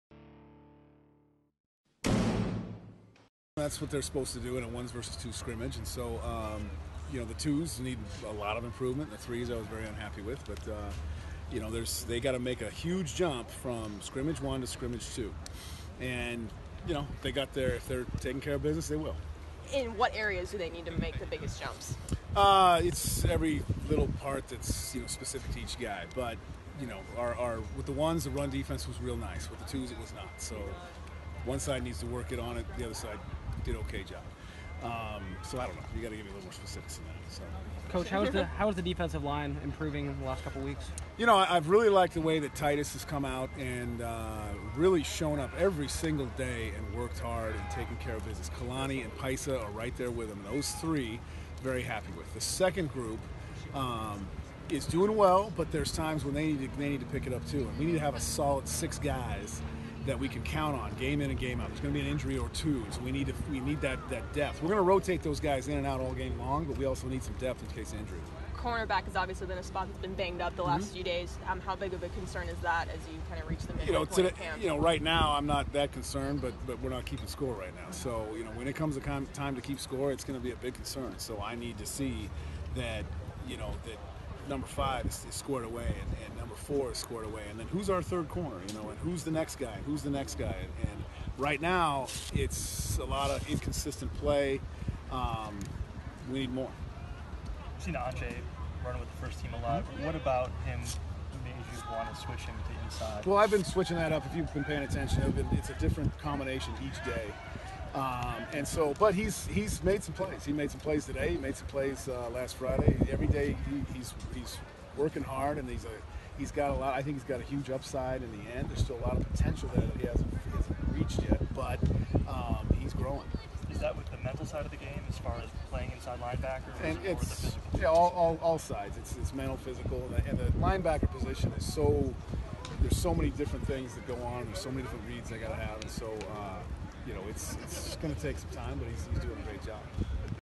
DC Kevin Clune gives defense update
Midway through the Beavers 2017 Fall Camp session, defensive coordinator Kevin Clune gives his thoughts on the defense.